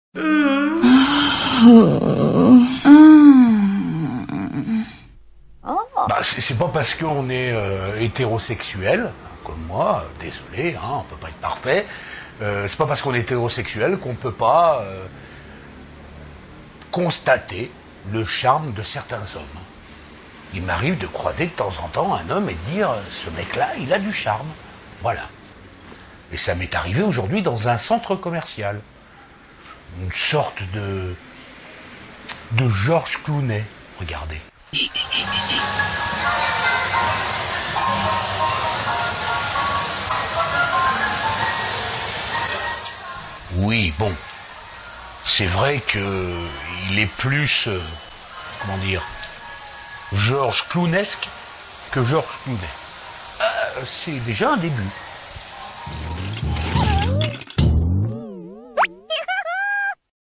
J'ai croisé Georges Clooney !!!
J'ai croisé Georges Clooney aujourd'hui ! Enfin...je ne sais pas si c’était vraiment lui ???